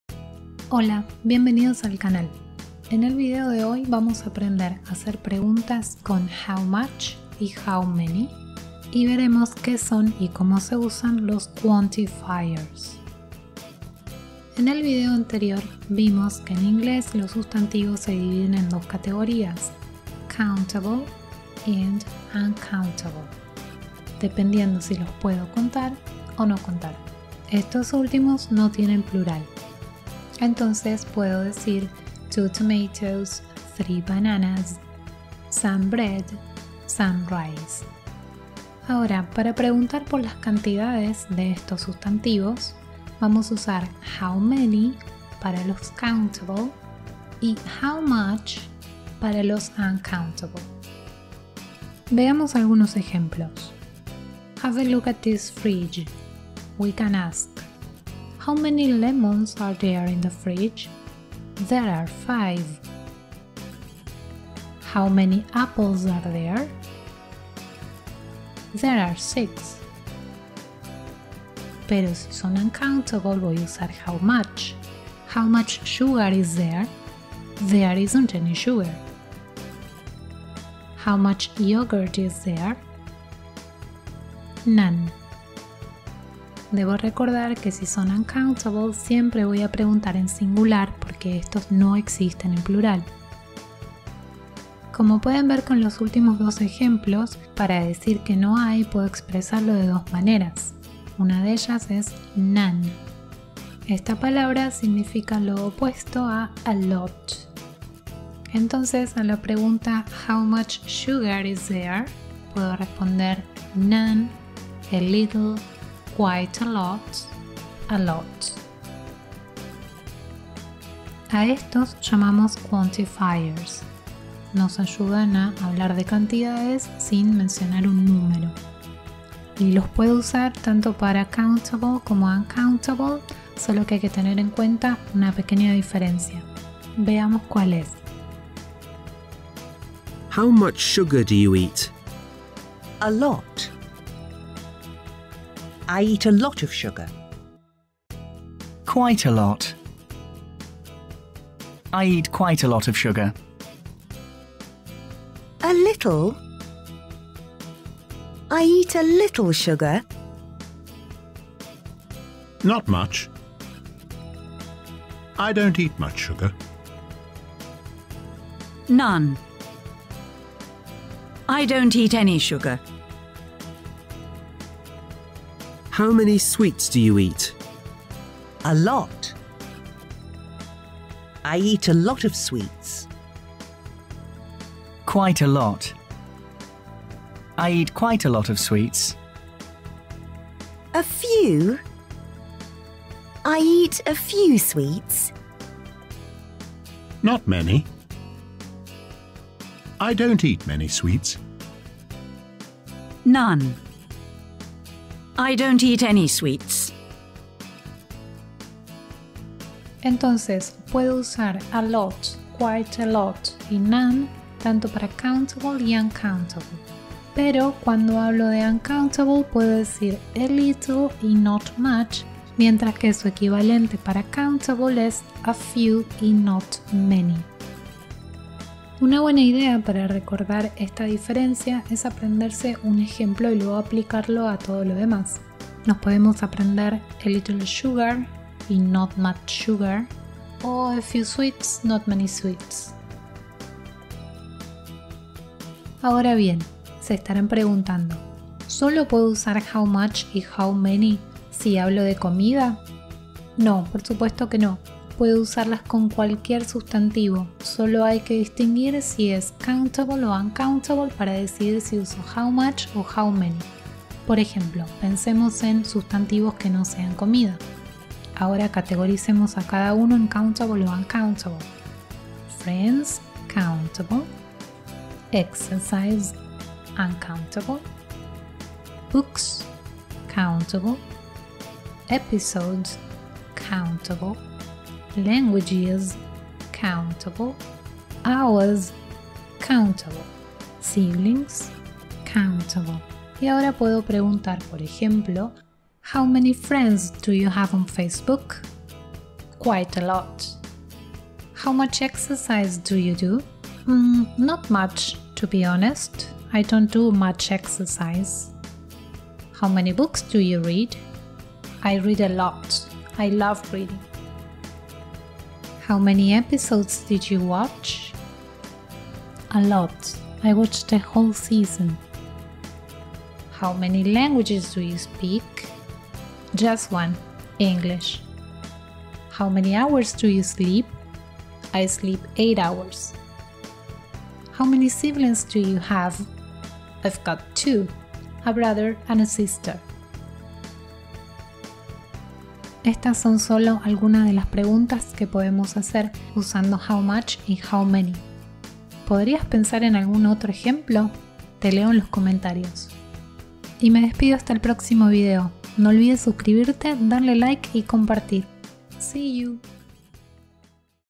How much? or How many?/Quantifiers/Countable & Uncountable nouns. Class 16
Capítulos en este video:
00:00 intro
00:16 countable and uncountable
00:43 how many and how much
02:00 quantifiers
04:05 using examples for everyday situations

Usamos How much..? y How many..? para preguntar  sobre cantidades. How much se usa con uncountable nouns (no se pueden contar y nunca llevan plural, como el azúcar = sugar) y How many con countable nouns, (se pueden contar como las manzanas = apples) por ej.:
How much sugar is there?
How much butter do you need to make a cake?
How much exercise do you do?
How much water do you drink a day?

How many calories are there in a chocolate bar?
How many hours do you sleep?
How many potatoes are there?

Y puedo responder con una cantidad especifica: 'There are 4 potatoes.' o sin mencionar un numero, puedo decir:
A lot./ Quite a lot. / None (for countable and uncountable nouns)
A little./ Not much. (only for uncountable nouns)
A few. / Not many. (only for countable nouns)

Te recomiendo este video que explica detalladamente cuales son los sustantivos Countable and Uncountable en inglés: